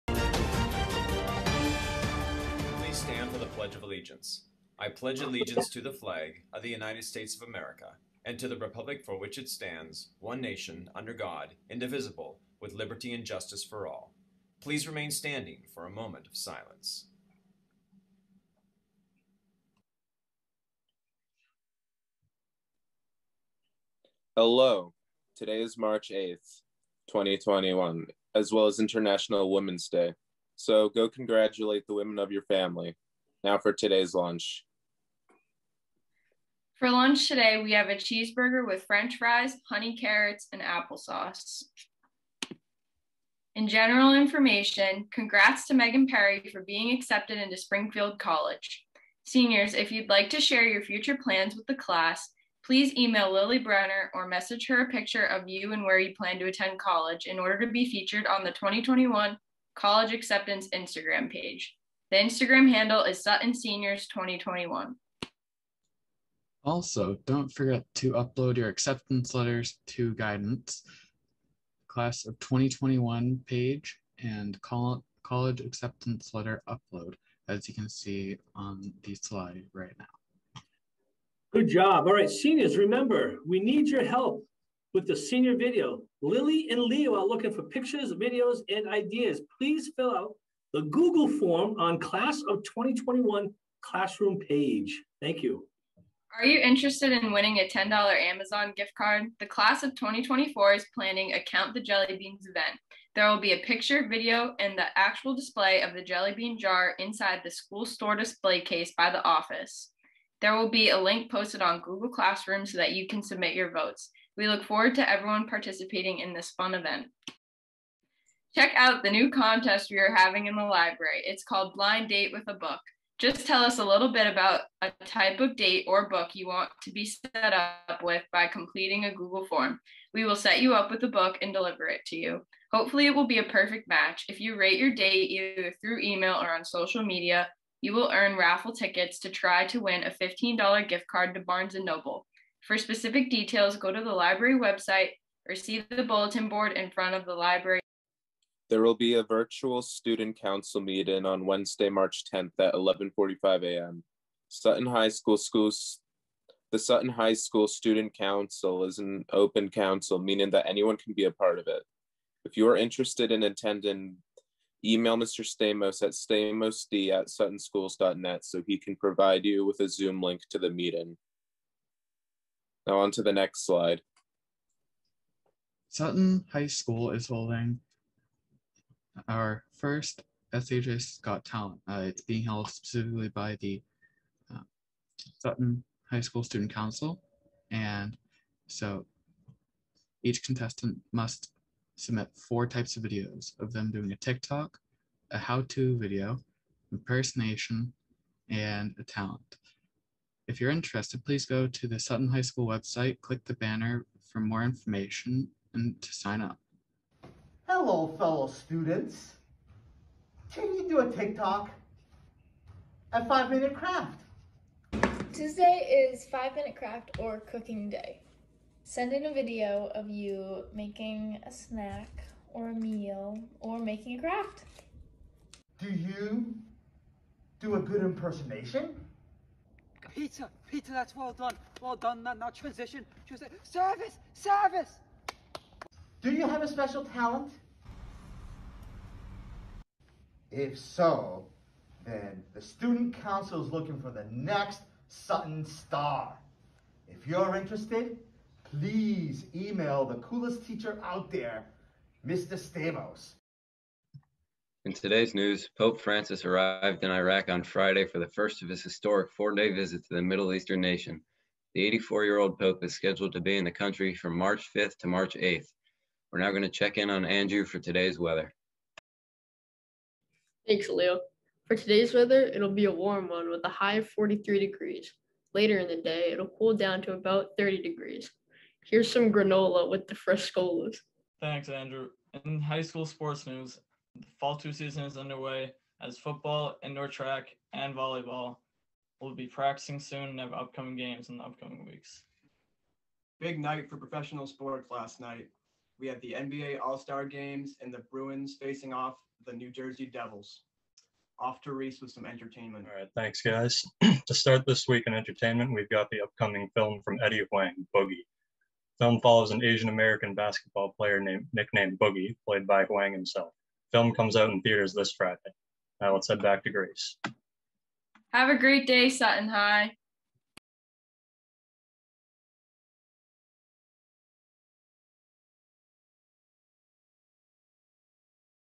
Morning Announcements Monday March 8, 2021
Today is International Women’s Day

Lunch Menu
Cheeseburger with French Fries Honey Carrot Coins Applesauce
Guidance News

Lily Brenner has kindly offered to host an Instagram page for college announcements.
If you’d like to share your future plans with the class, please email  or message her a picture of you and where you plan to.
Instagram handle is: suttonseniors2021

Also, don’t forget to upload your acceptance letters to guidance
Class of 2021 to  College Acceptance Letter Upload
General Information

SENIORS:   Please we need you to help us with the senior video.  Lily and Leo are looking for pictures, videos and ideas, please fill out the google form on the class of 2021 classroom page.

Are you interested in winning a $10 Amazon gift card? The class of 2024 is planning a "Count the Jelly Beans" event! There will be a picture, video and the actual display of the jelly bean jar inside the School Store display case by the office (located in front of the office). There will be a link posted on Google Classroom so that you can submit your votes. We look forward to everyone participating in this fun event.

Athletics
Indoor track will run from March 15-April 26th.  If you are interested in preparing for Spring season - register NOW on FamilyID or see Ms. Tuomala at lunch.  Registrations will need to be completed by Thursday, March 11th with updated physicals on file with the Health office.

Other
Check out the new contest we are having in the library. It’s called Blind Date With a Book.
Just tell us a little bit about a type of date (or book) you want to be set up with by completing a Google form. We will set you up with a book and deliver it to you. Hopefully it will be a perfect match! If you rate your date, either through email or on social media, you will earn raffle tickets to try to win a $15 gift card to Barnes and Noble! For specific details go to the library website or see the bulletin board in front of the library entrance.

There will be a virtual student council meeting on Wednesday March 10th at 11:45am. Sutton High School Student Council is an open council meaning that anyone can be part of it. If you are interested in attending email Mr. Stamos at so he can provide you with a zoom link to the meeting.

 Also: News, Weather, and Sports with the Frascolla's

Have a great day Sutton High!